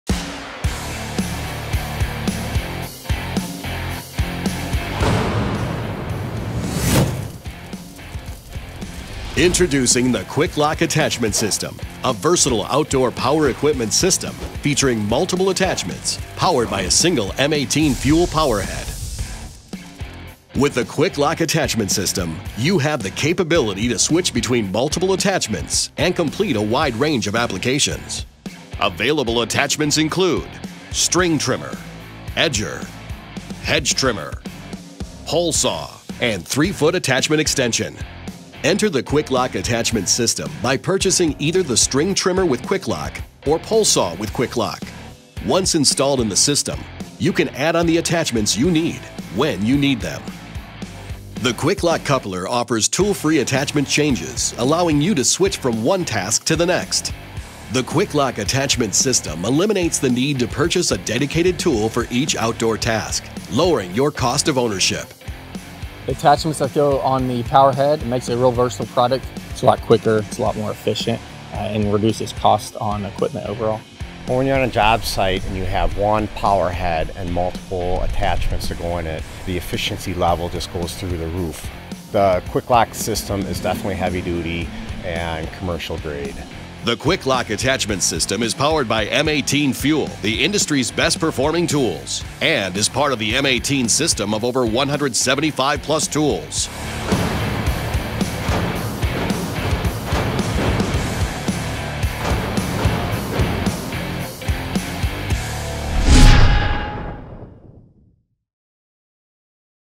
Milwaukee® M18 FUEL™ QUIK-LOK™ Attachment System
Designed to meet the needs of landscape maintenance professionals, the QUIK-LOK™ Attachment System is a versatile outdoor power equipment system featuring multiple attachments powered by a single M18 FUEL™ Power Head. With the QUIK-LOK™ Attachment System, you have the capability to switch between multiple attachments including a string trimmer, edger, hedge trimmer and pole saw to complete a wide range of applications. Powered by M18 FUEL™, the QUIK-LOK™ Attachment System utilizes the most advanced cordless technology to deliver best-in-class power and run-time for landscaping and trade professionals, achieving instantaneous throttle response and outperforming higher voltage platforms.